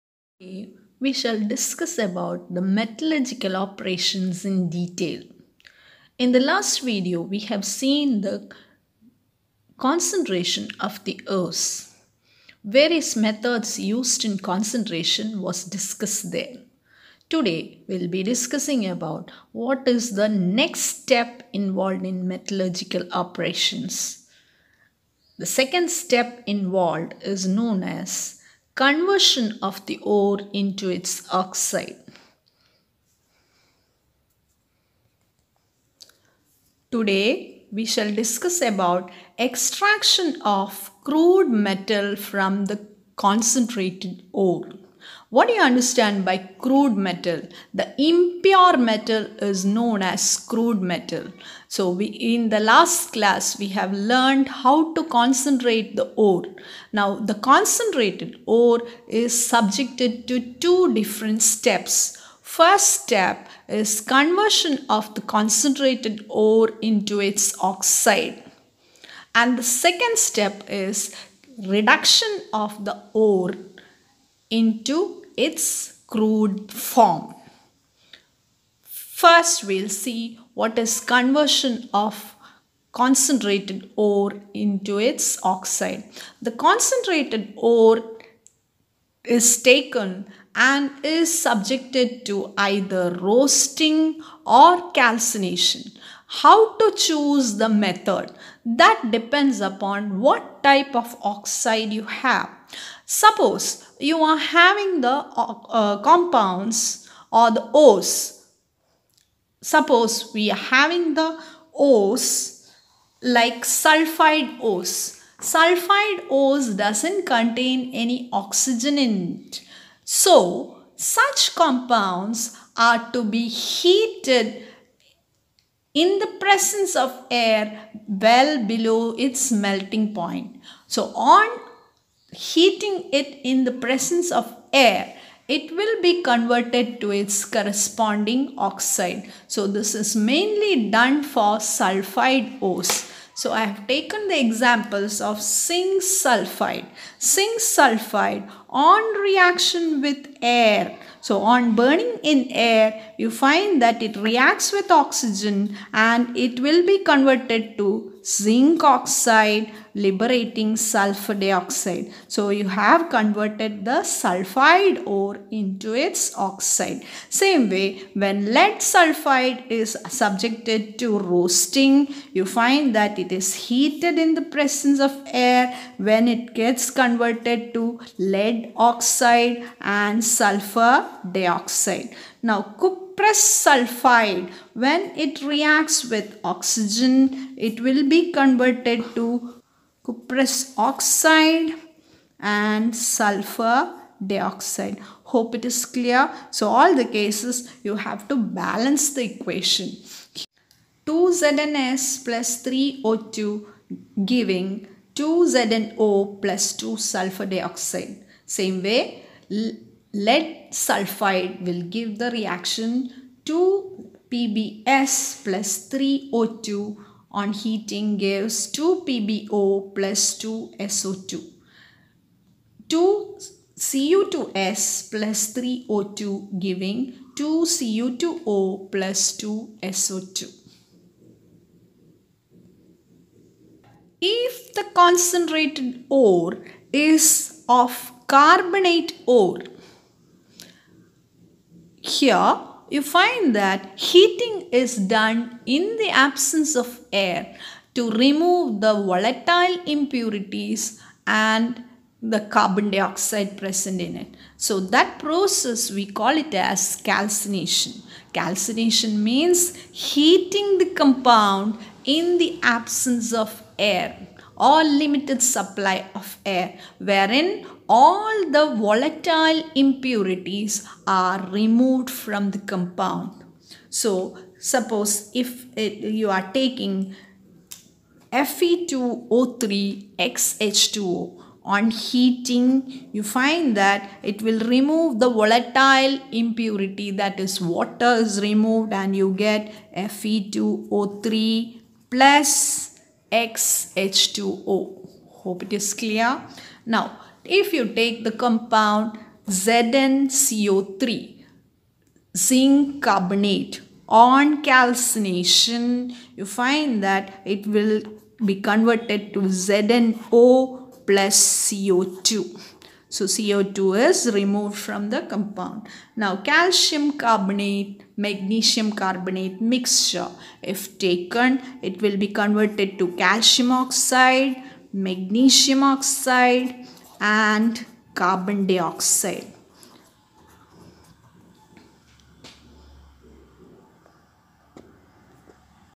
Class xii Metallurgy- Part2 Conversion of ore into its oxide
Processes  of roasting & calcination is discussed  in detail in this video.